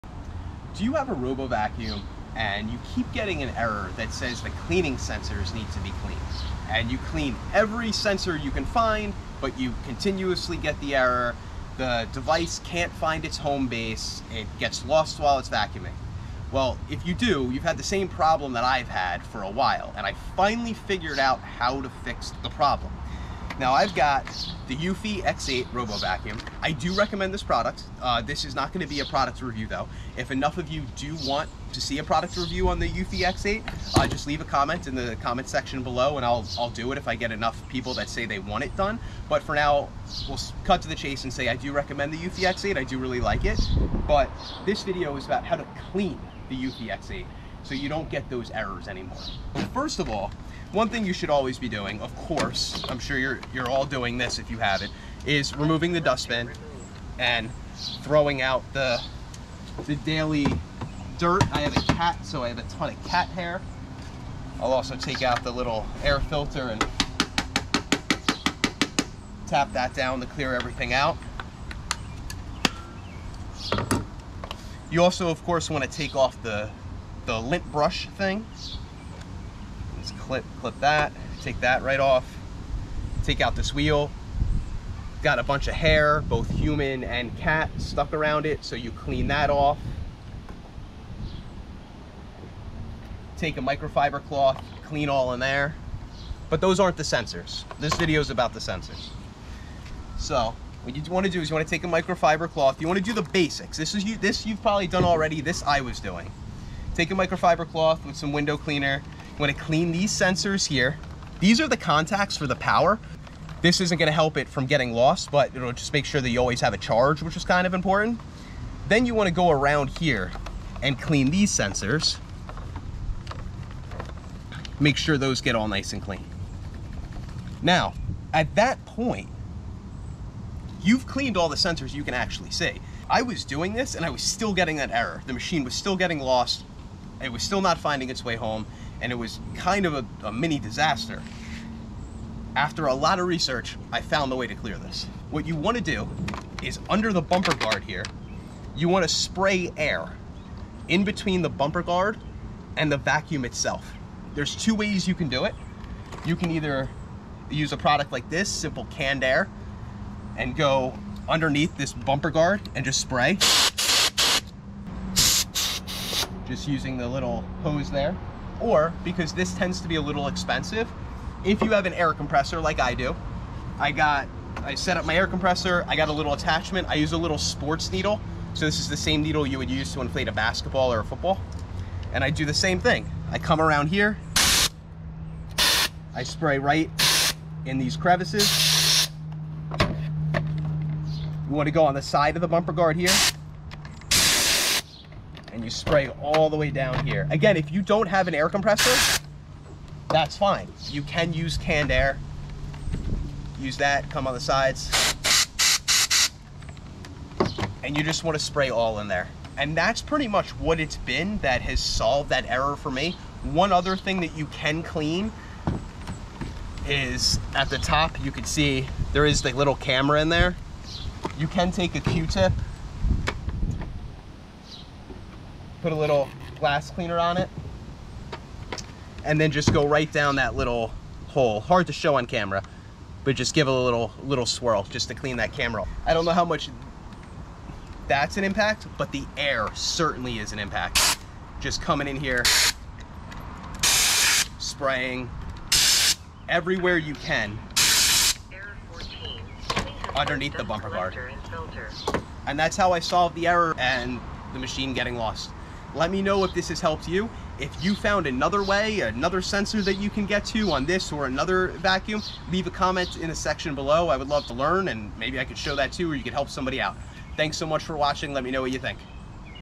How to clean robot vacuum sensors to clear errors and prevent lost robot
In this video I show how to clean the Eufy X8 robot vacuum. Most likely this same type of cleaning will also be helpful on all robot vacuums. If your robot vacuum gets lost while cleaning or if it cannot find its way home this video is for you. If your vacuum gives you an error telling you to clean the sensors, this video is for you.